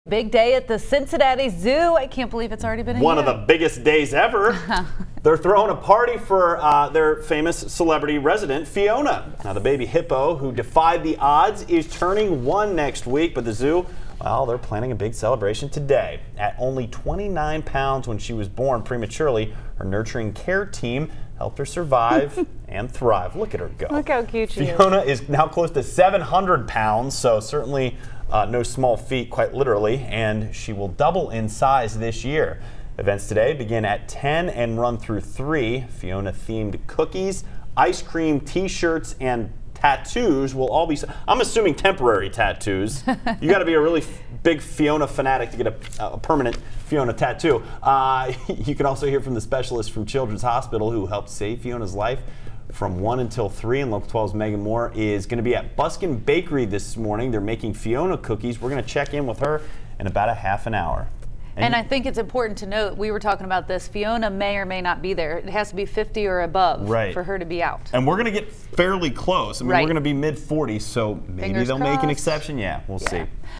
Cincinnati Zoo celebrates Fiona's 1st birthday
CINCINNATI (WKRC) - Saturday was a big day at the Cincinnati Zoo.

The zoo threw a party for its resident celebrity, Fiona. The baby hippo who defied the odds is turning 1 next week.

At only 29 pounds when she was born prematurely, Fiona's nurturing care team helped her survive and thrive. She's now close to 700 pounds, and she will double in size in 2018.

The festivities kicked off at the zoo on Saturday with Fiona enjoying some birthday cake.

Fiona-themed cookies, ice cream, t-shirts and tattoos were available at the celebration, and patrons were able to hear from the specialists from Children's Hospital who helped save Fiona's life.